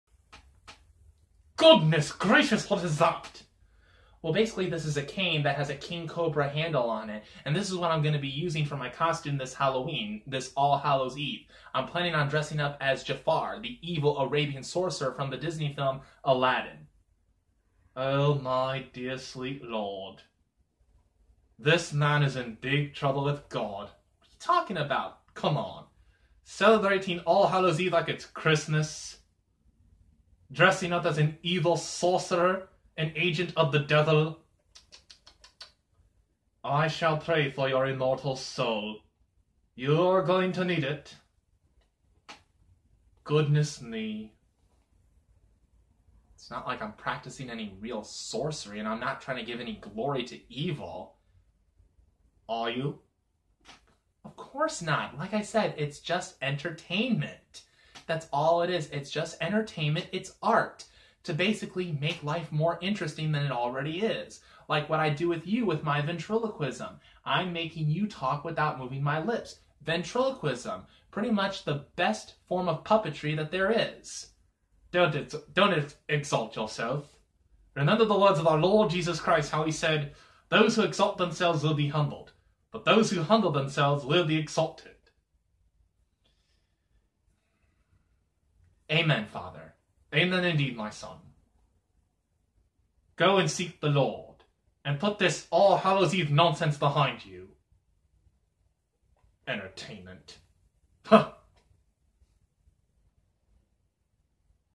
St. Patrick Doesn't Like Halloween
St. Patrick makes it pretty clear that Halloween is nothing more than an ungodly and unholy holiday that celebrates evil when I tell him what my plans are for Halloween.